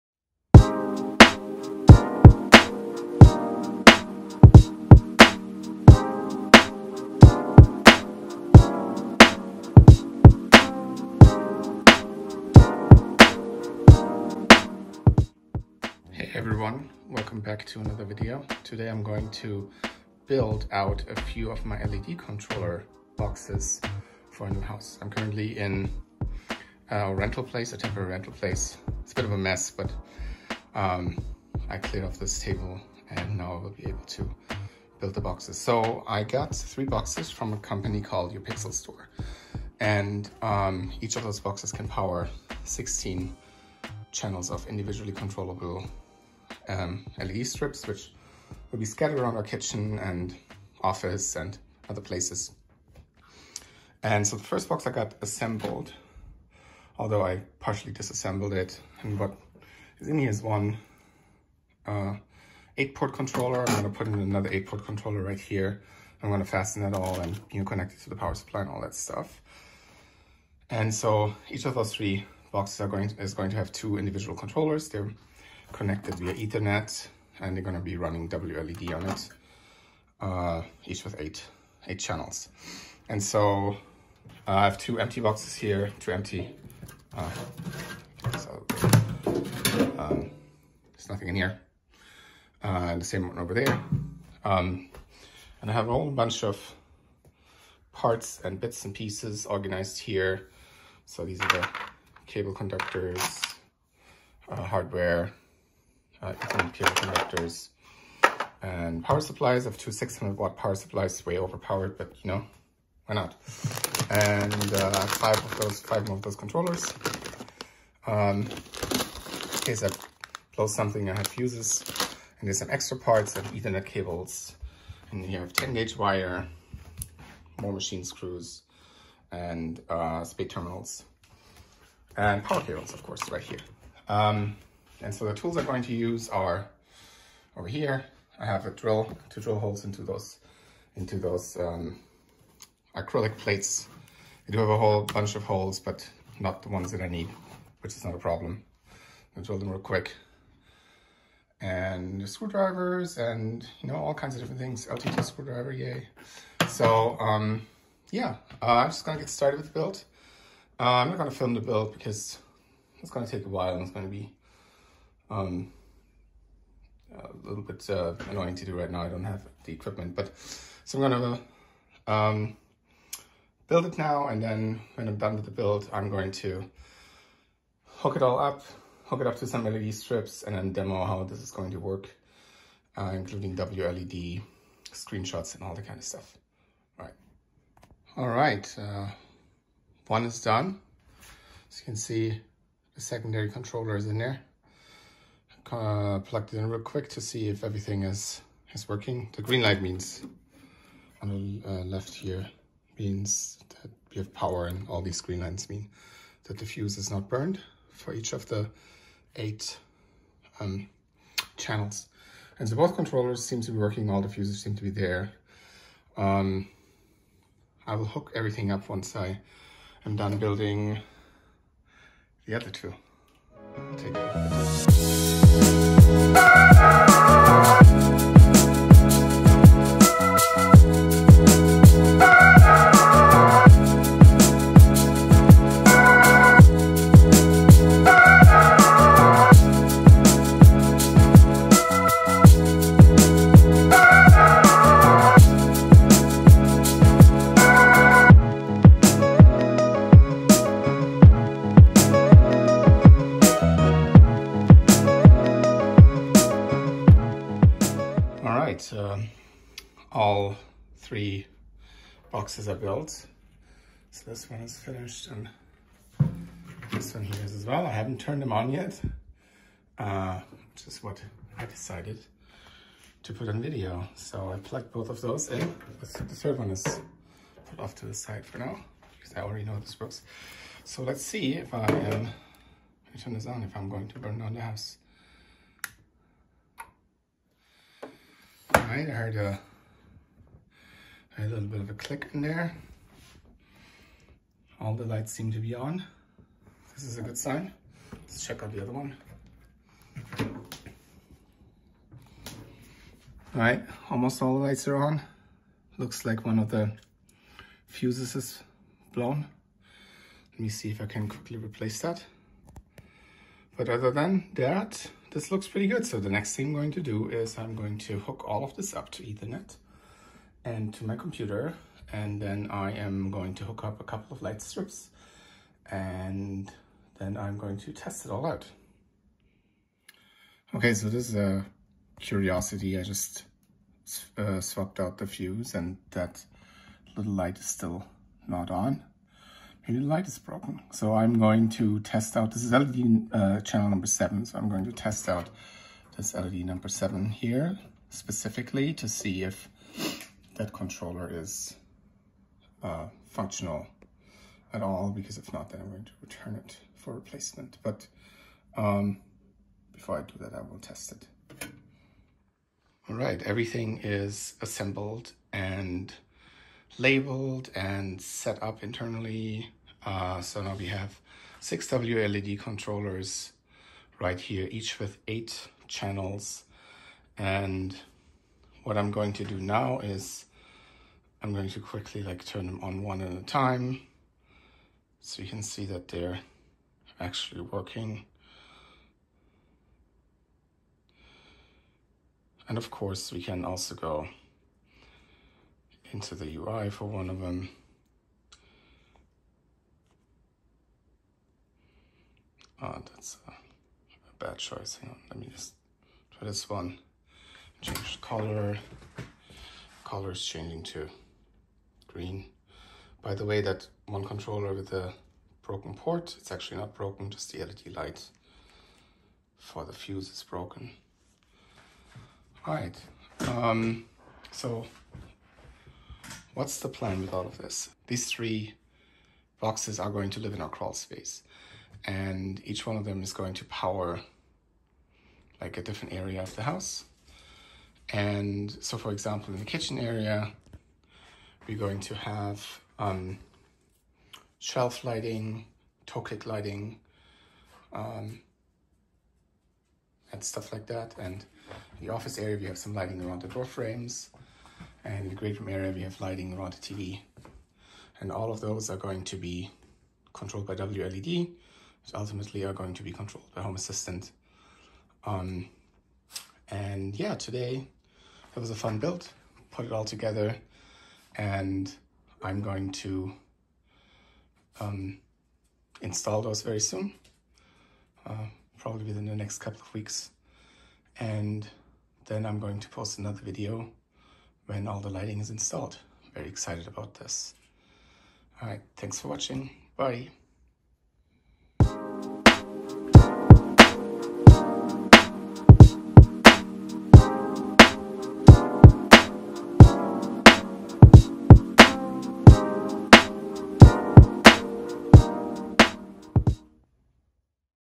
WLED Controller Build
In this video, I'm assembling and testing multi-channel WLED controllers that I got There's a total of 6 controllers, and each of the controllers can power up to 8 channels of individually controllable LED strips, giving a total of 48 channels. These channels will power LED strips in our kitchen, great room, office, and other areas.

Timestamps:
0:00 Intro
0:15 What's the plan?
3:07 First box done
3:45 Box two and three build
4:18 Will I burn down the house?
6:02 LED lights in action
7:07 Lookahead
8:11 Outro